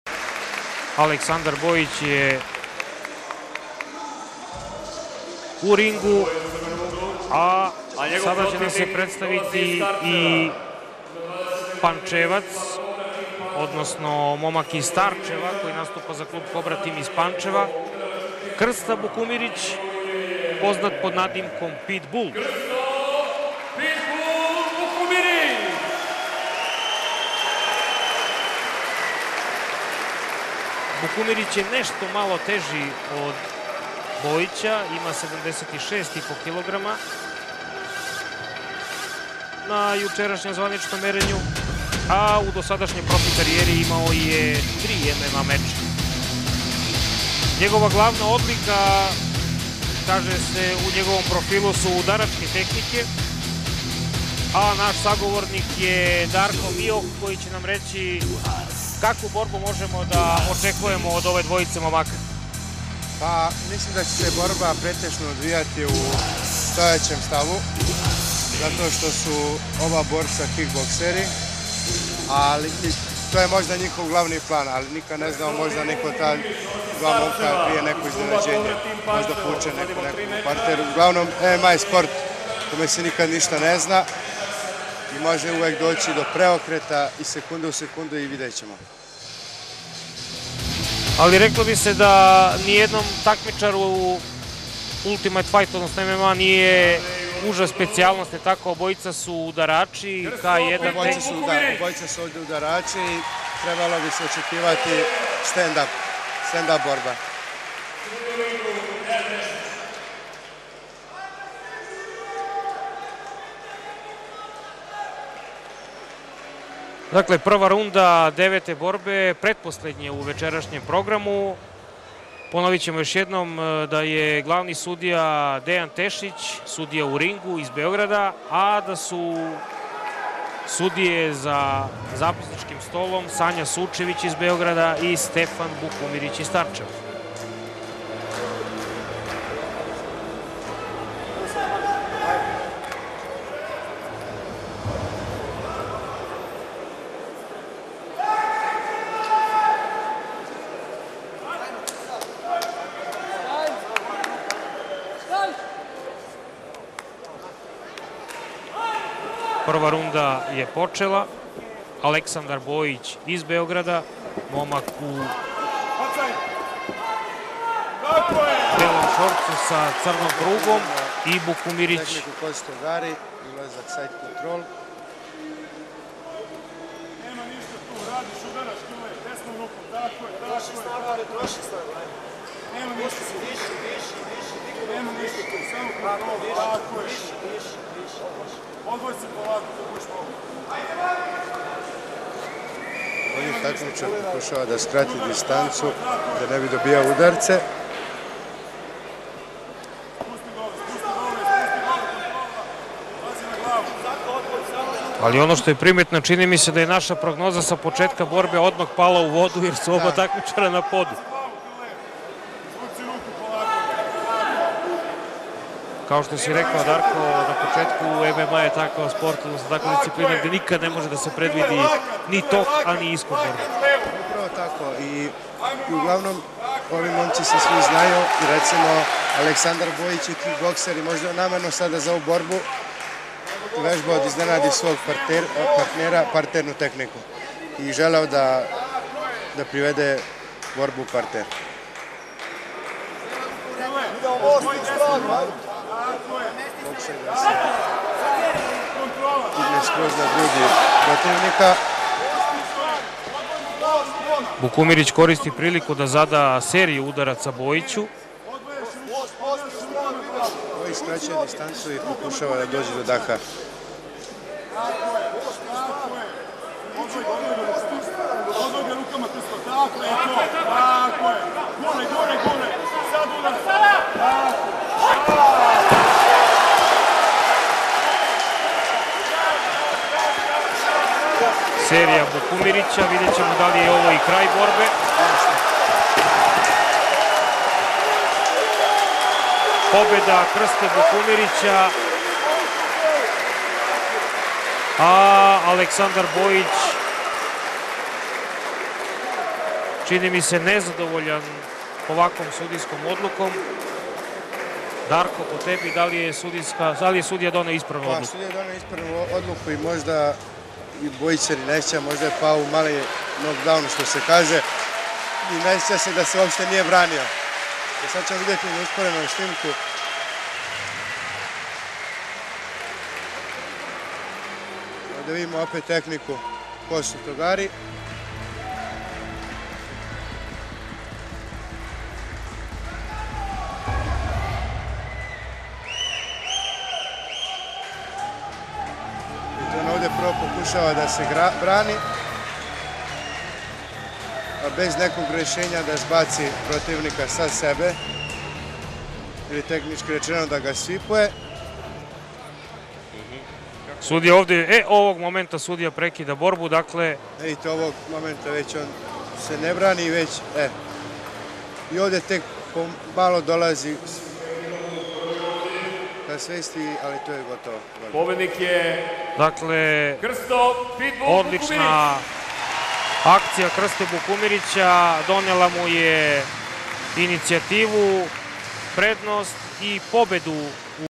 Krsto Bukumiric vs Aleksandar Bojic Combat Challenge 5
Borilacki spektakl koji se odrzao u Vrscu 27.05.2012